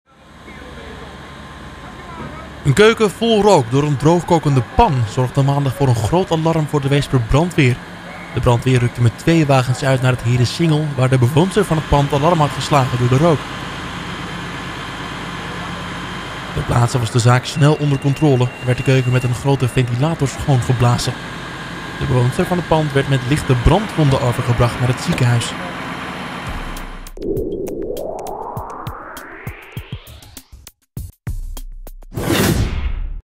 Bewoonster gewond na keukenbrand
WEESP - Een brandje in de keuken heeft maandagmiddag een woning aan de Herensingel in Weesp vol rook gezet. De bewoonster is met lichte brandwonden aan haar hand naar het ziekenhuis overgebracht. Ook had zij veel rook binnengekregen.

Omdat de situatie zich aanvankelijk ernstig liet aanzien (men vreesde voor een uitslaande woningbrand), rukte de brandweer met veel materieel uit.

Ter plaatse was de zaak snel onder controle. Vermoedelijk is alles veroorzaakt door een pan die stond 'droog te koken'. Met behulp van een grote ventilator is alle rook uit het huis geblazen.